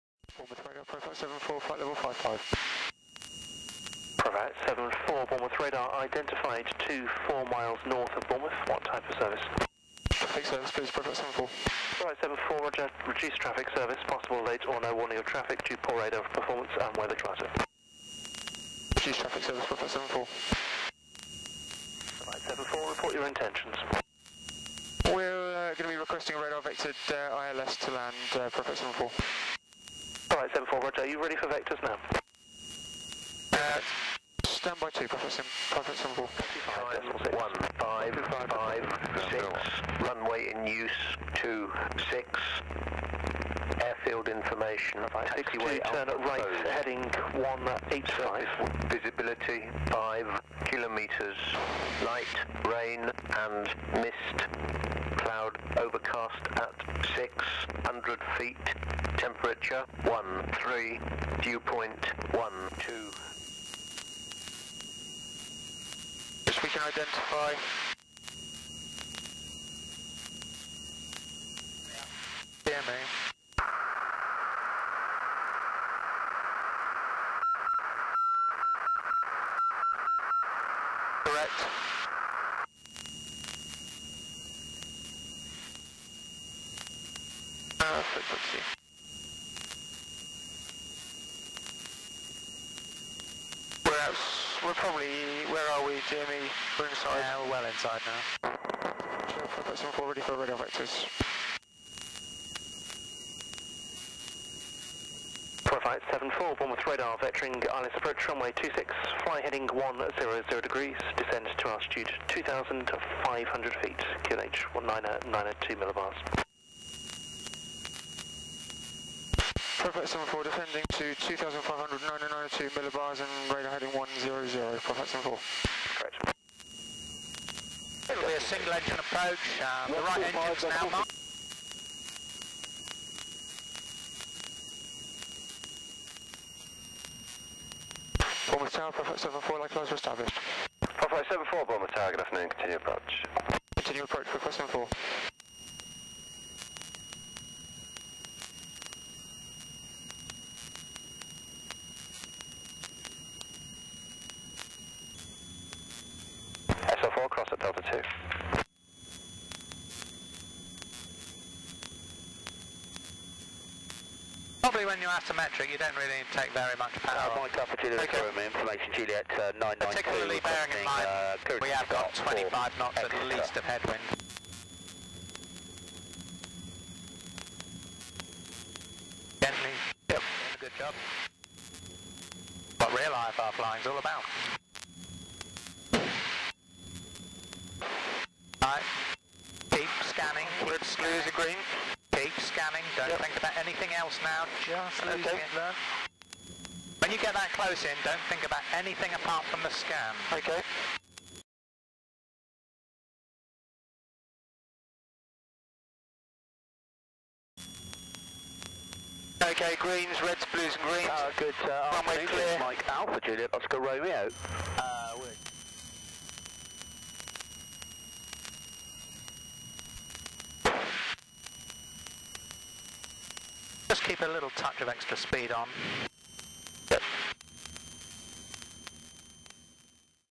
Radar vectored Asymmetric ILS Bournemouth EGHH followed by low level bad weather circuit
Radar vectored ILS Bournemouth EGHH followed by Asymmetric go-around & low level bad weather circuit, runway 26, Professional Air Training, PAT, Beech Duchess, BE76